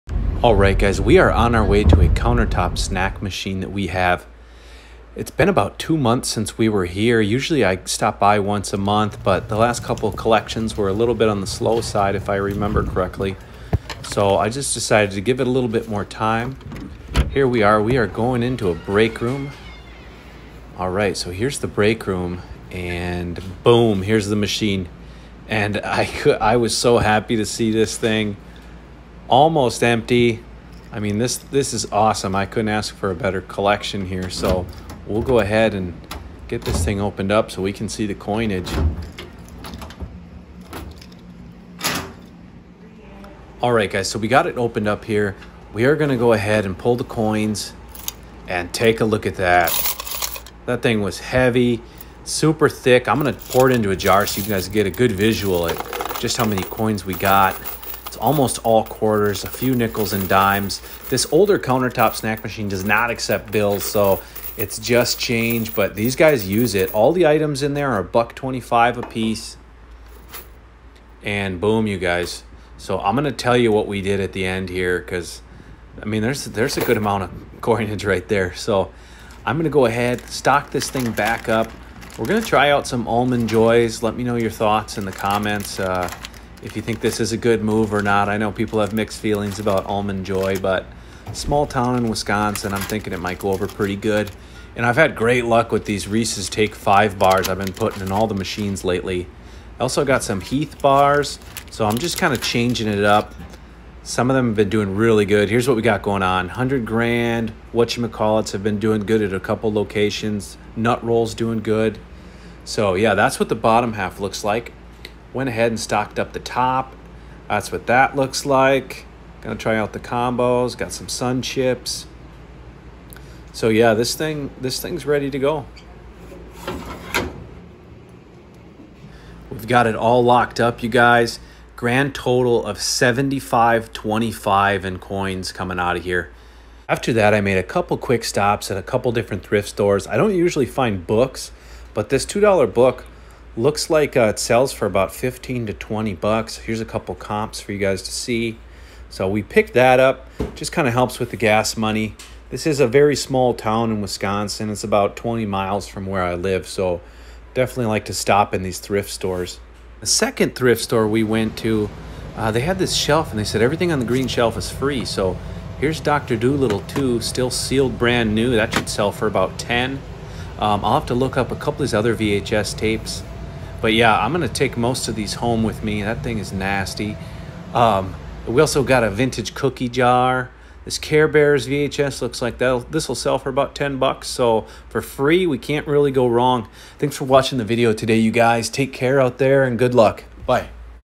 Near EMPTY Machine - Vending Collection $$$ 🙂 🪙 Plus Items
This vending machine was almost empty! Money Collection. Countertop snack machine SEAGA 3000
- Top line thrift find